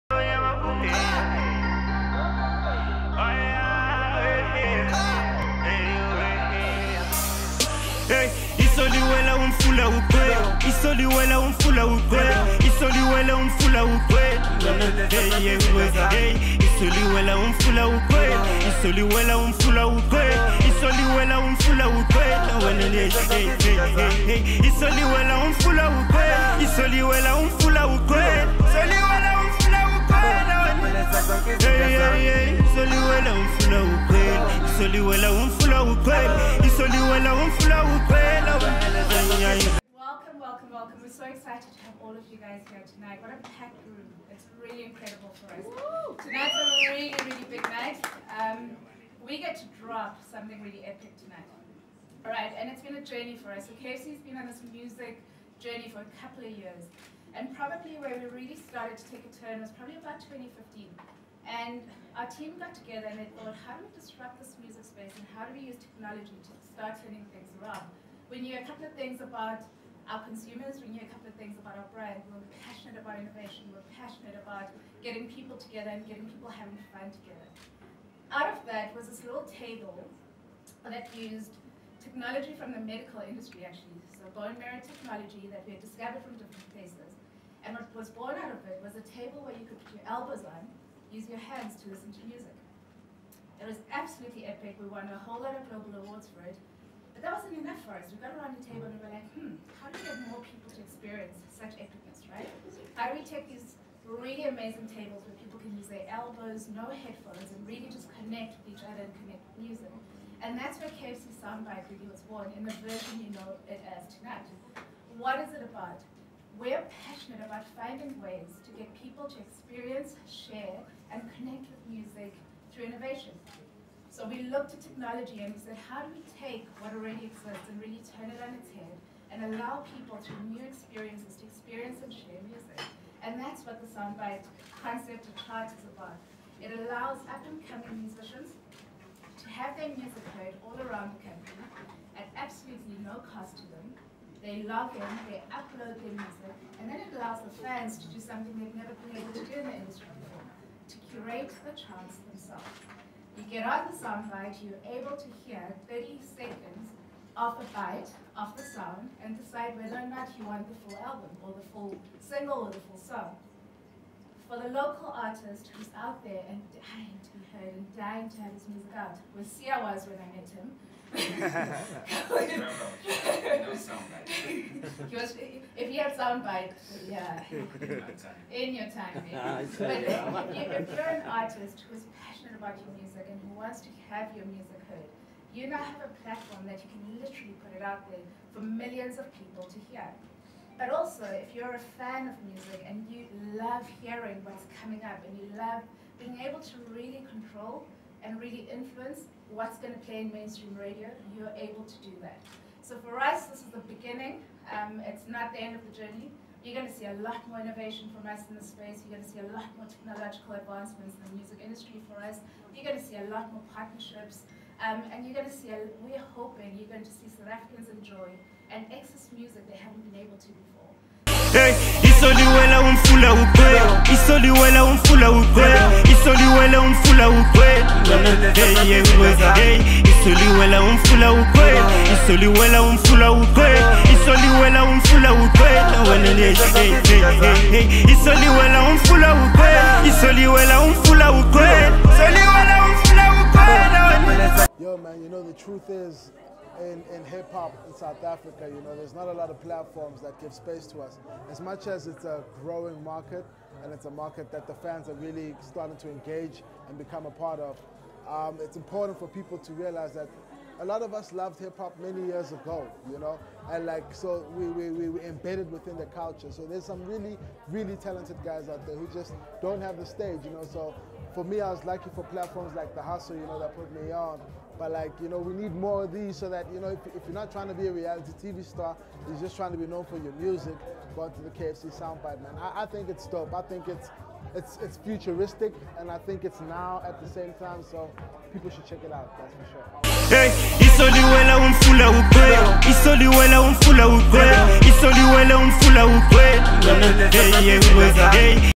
KFC’s Innovative ‘SoundBite’ Technology Promotes Emerging Local Musicians
With the increasing emergence of local musicians came a way to engage the brand with its target audience by introducing them to unsigned local artists. Installed in the KFC Willows store in Bloemfontein, the ‘KFC SoundBite’ technology is demonstrated in the case study above.

The in-store technology includes an interactive record table with innovative ‘bone technology’. Using this, customers are able to listen to local sounds simply by placing their elbows on the audio tables – giving them a mesmerizing musical experience without disturbing anyone else in the store. Directional sound speakers are distributed through the store to give customers sitting in booths the ability to customise their music experience by choosing from a vast selection of DMX content via their cellphones. USB charging stations are conveniently situated at tables and screens are used as an interactive point of sale to stream daily specials and content.

On the music component, KFC worked with local artists Lebo Lukewarm, Ginger Breadman, 2Lee Stark as well as Priddy Ugly to launch the SoundBite campaign. While gaining support for local artists, the activation aims to increase return visits to KFC stores by reminding customers that it is still one of the coolest food outlets around.